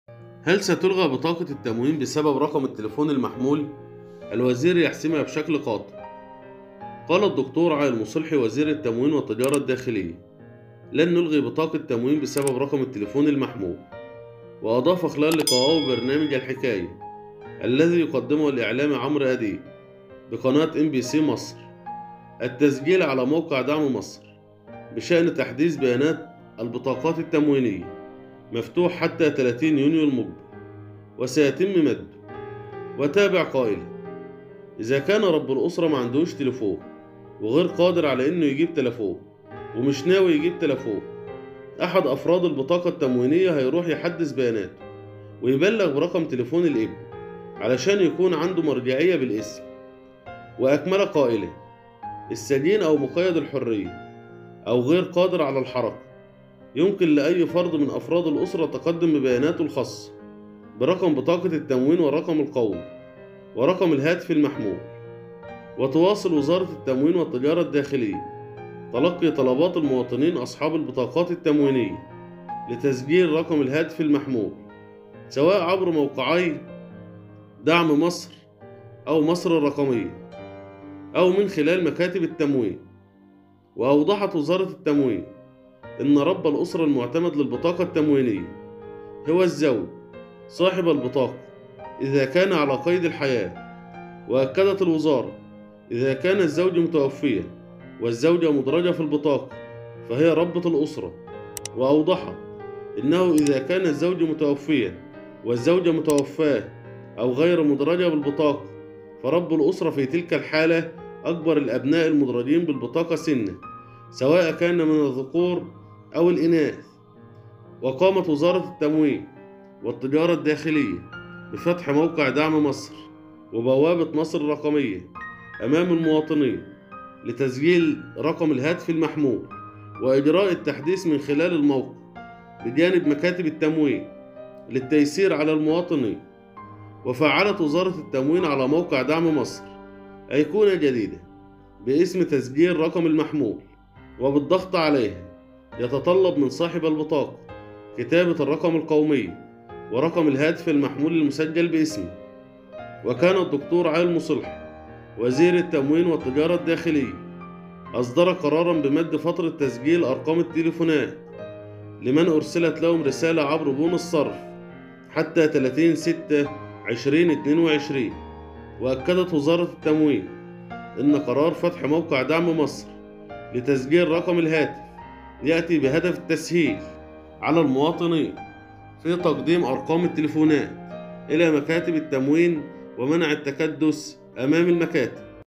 عاجل الآن مد فترة تسجيل الهاتف لبطاقة التموين ووزير التموين يجيب لو مسجلتش الهاتف او ممعكش تليفون
مد فترة تسجيل رقم هاتف لبطاقة التموين
وزير التموين والتجارة الداخلية يجيب
وزارة التموين تحذف ثلاث فئات من البطاقات التموينية خلال الشهر الجاري بطاقات التموين تحديث بطاقات التموين